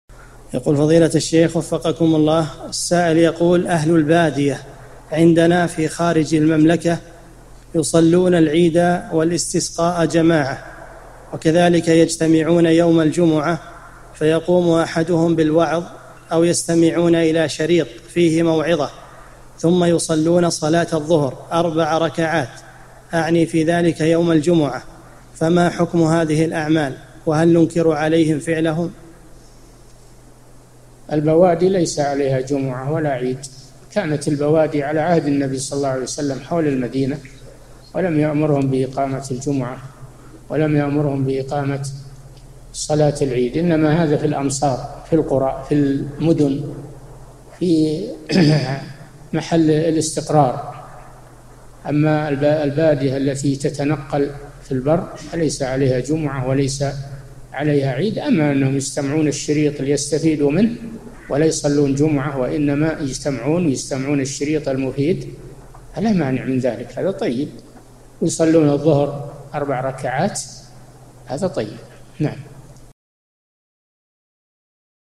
[758 /1350] أهل البادية عندنا يجتمعون أيام الجمع فيستمعون إلى شريط فيه موعظة ثم يصلون الظهر..
-[758 /1350] السؤال: أهل البادية عندنا في خارج المملكة يصلون العيد والاستسقاء جماعةً، وكذلك يجتمعون يوم الجمعة فيقوم أحدهم بالوعظ أو يستمعون إلى شريطٍ فيه موعظة ثم يصلون صلاة الظهر أربع ركعاتٍ، أعني في ذلك يوم الجمعة، فما حكم هذه الأعمال؟ وهل ننكر عليهم فعلهم؟
 الجواب: البوادي ليس عليها جمُعة ولا عيد، كانت البوادي على عهد النبي صلى الله عليه وسلم حول المدينة، ولم يأمرهم بإقامة الجمُعة، ولم يأمرهم بإقامة صلاة العيد، إنما هذا في الأمصار، في المدن، في محل الاستقرار. أما البادية التي تتنقل في البر، فليس عليها جمُعة وليس عليها عيد.أما إنهم يستمعون الشريط ليستفيدوا منه، ولا يُصلون جمُعة وإنما يجتمعون يستعمون الشريطَ المُفيد، فلا مانع من ذلك، هذا طيب، ويُصلونَ الظهر أربع ركعات، هذا طيب.
للاشتراك في قناة الشيخ صالح الفوزان
أسئلة وأجوبة من شرح قاعدة جليلة ( 1350 ) مقطع

للاستفسار والإقتراحات والدعم
رئيس الفريق
لمشروع كبار العلماء